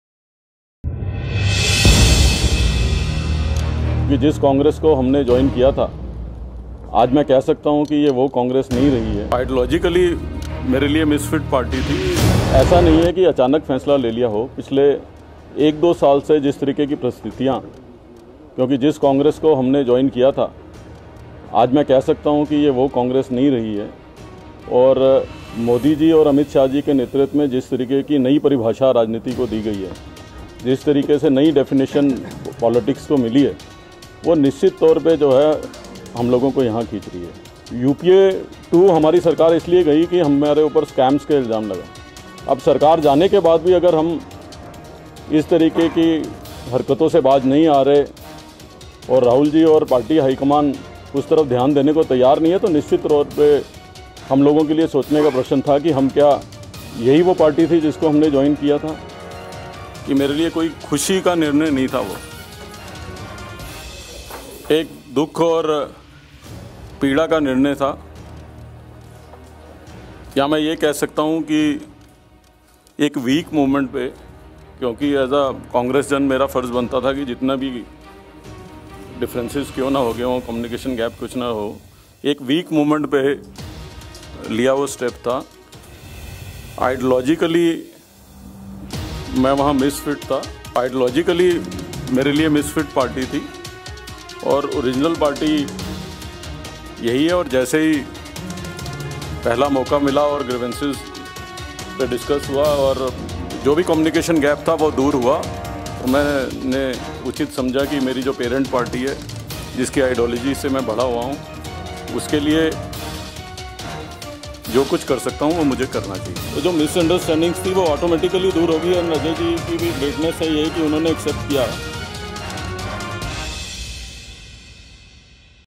कैसे बदल गए लवली के सुर !
नौ महीने पहले मोदी और शाह की की थी तारीफ़, अब बता रहे है खुद को भाजपा में मिसफिट।